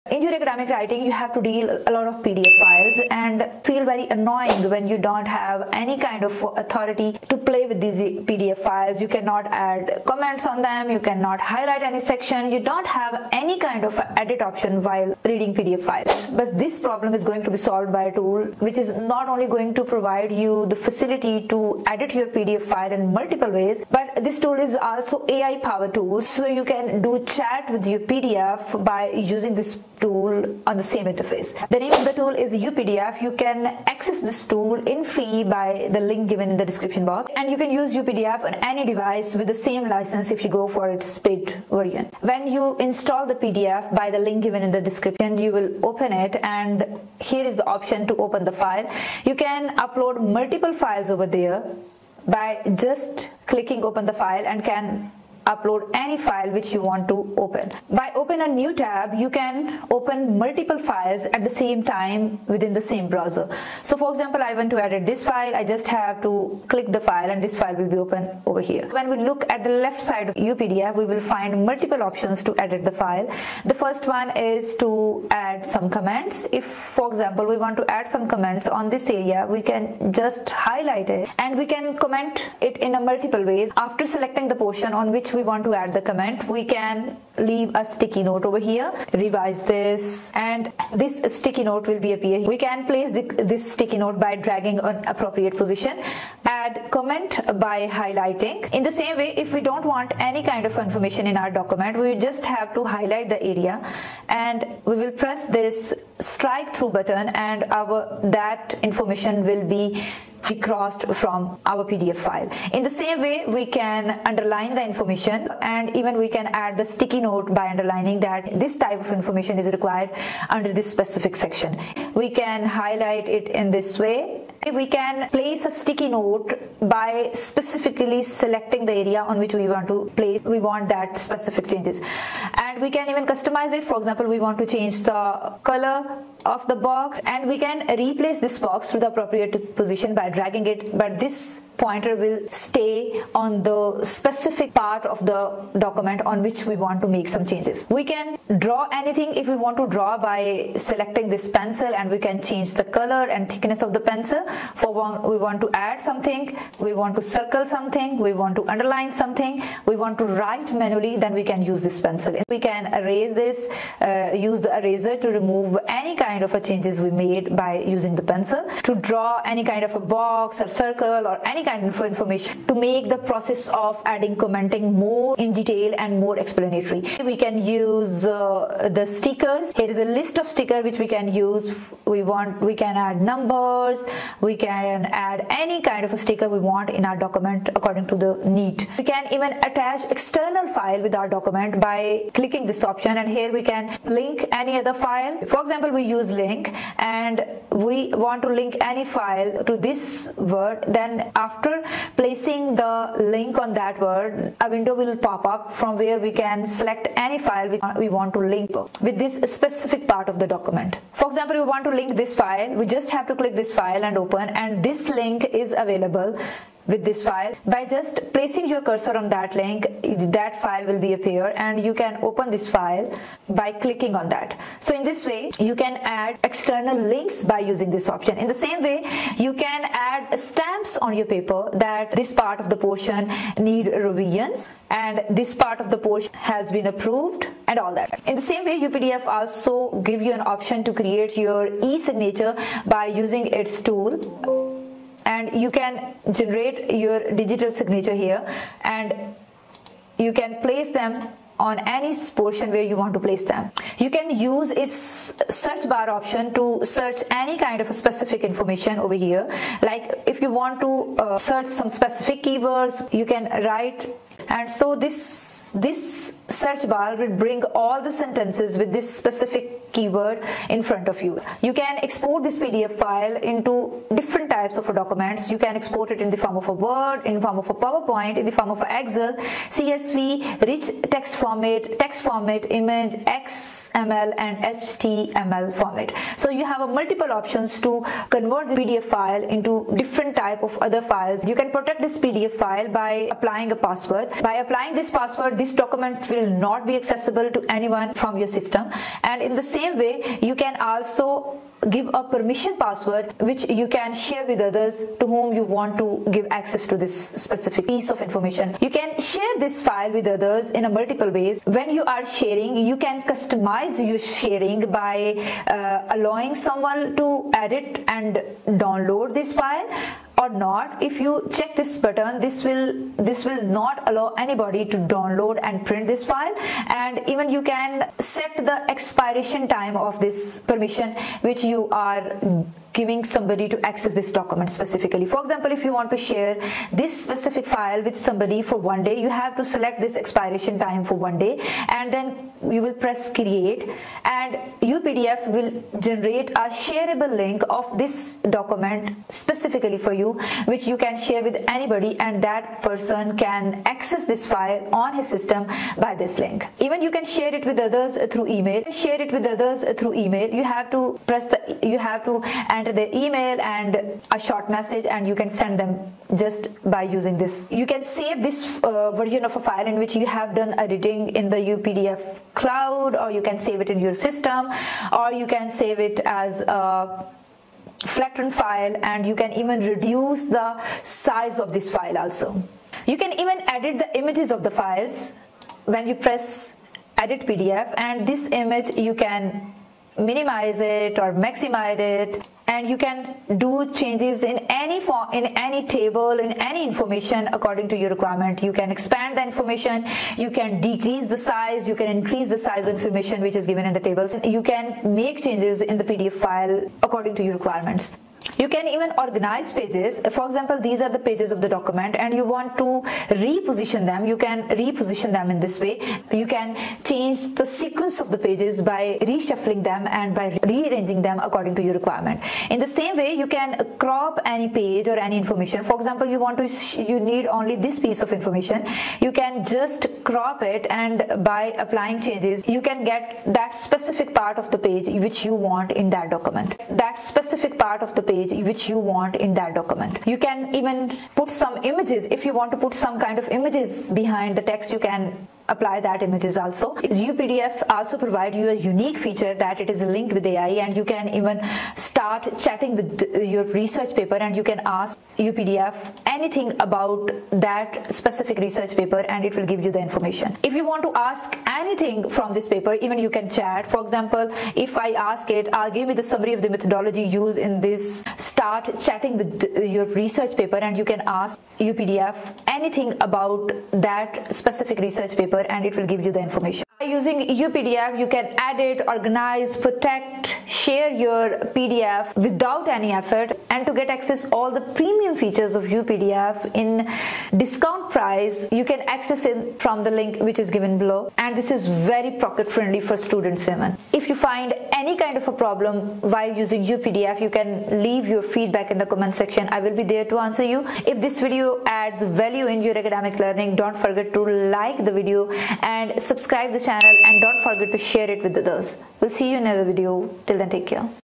Edit & Chat with PDF: Explore the Incredible AI Capabilities of UPDF
1. Get AI-powered UPDF Editor with an Exclusive Discount Now One License Can be used on All Platforms to Edit and Sync PDFs!
2.
3.

 Times Map
1:00 Introduction
0:43 How to upload files in UPDF
1:03 How to edit files in UPDF
4:32 How to change the file size and type in UPDF
4:46 How to protect the file  in UPDF
5;07 How to share the file in UPDF
4:32 How to change the file size and type in UPDF
6:20 How to edit image and tables in pdf with  UPDF
6:23 How to change the file size and type in UPDF
6:46 How to rearrange the pdf pages in  UPDF
7:27 Chat with pdf With Unique feature of UPDF

▼ ▽ Recomended AI Tools In Research